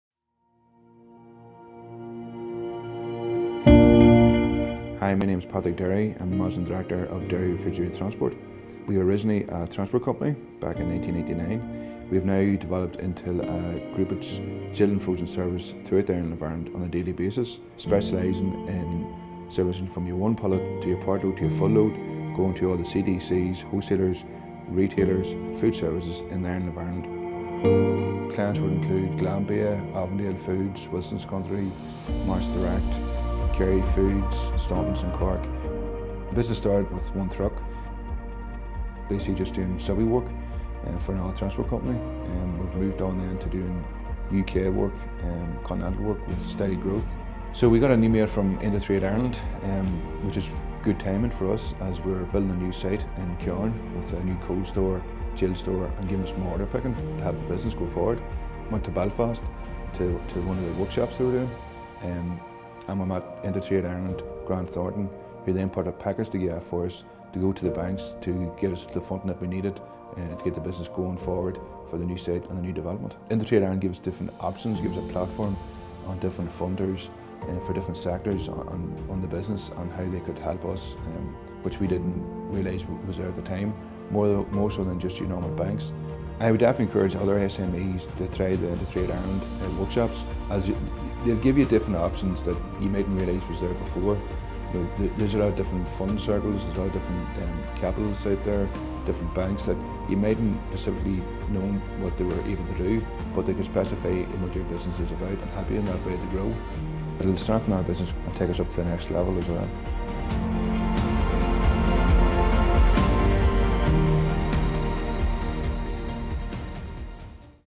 InterTradeIreland's Funding Advice Service
Find out how InterTradeIreland's Funding Advice Service has helped Derry Refrigerated Transport future proof its business